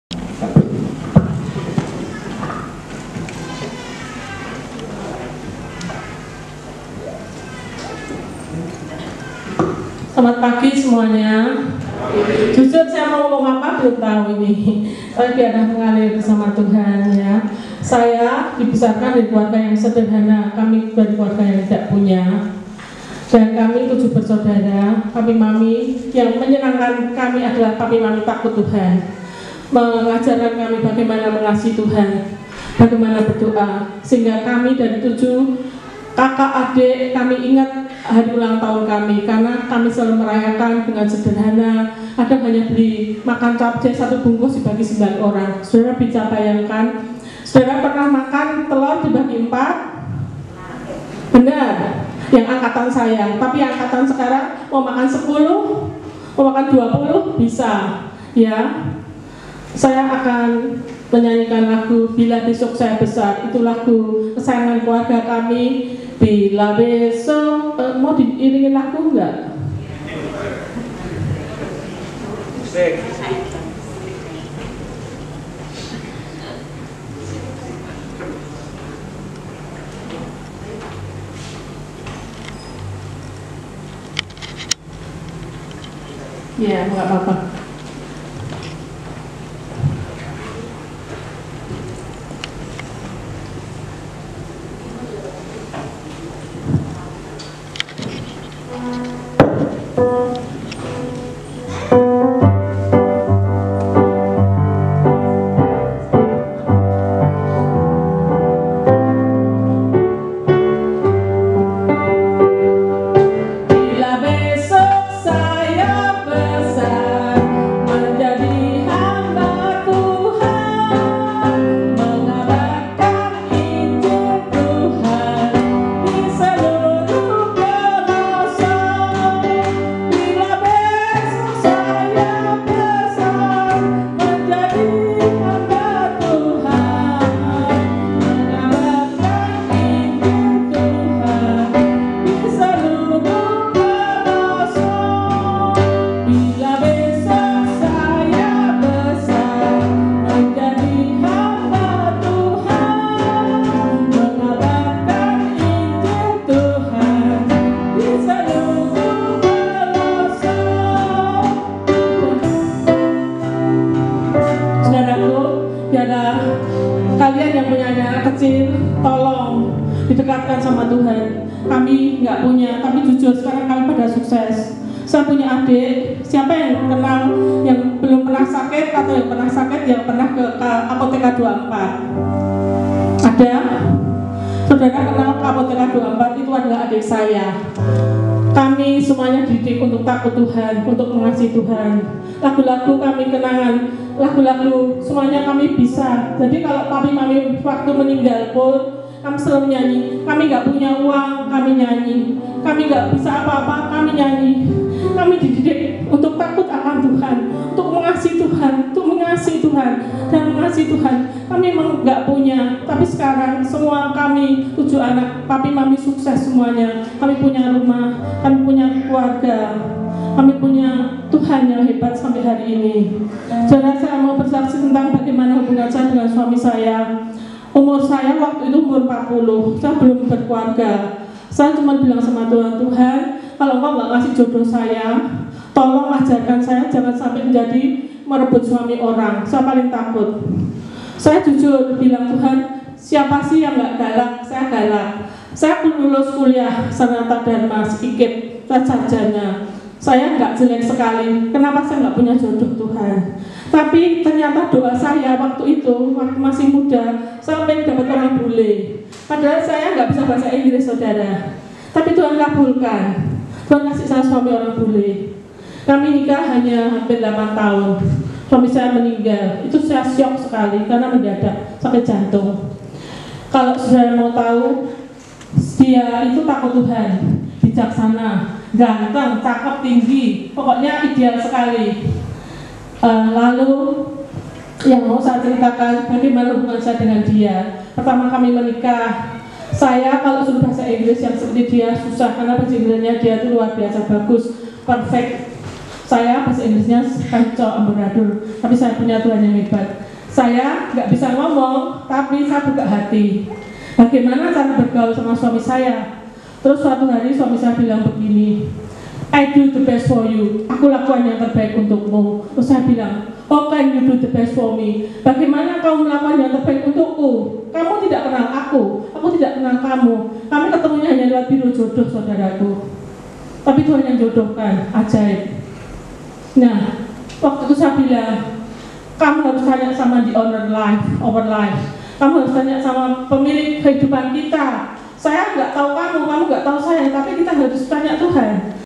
Video 1. June 12, 2022 || Ibadah Umum Pagi - GBI Galilea || Videographer : Blacktator.
Video 1. June 12, 2022 || Ibadah Umum Pagi - GBI Galilea (Gereja Bethel Indonesia "Galilea") || Location : GBI Galilea - Jl. Letjen Suprapto, Gang M Ali III No. 9, Tanah Tinggi, Johar Baru, Jakarta Pusat, Indonesia || Videographer : Blacktator.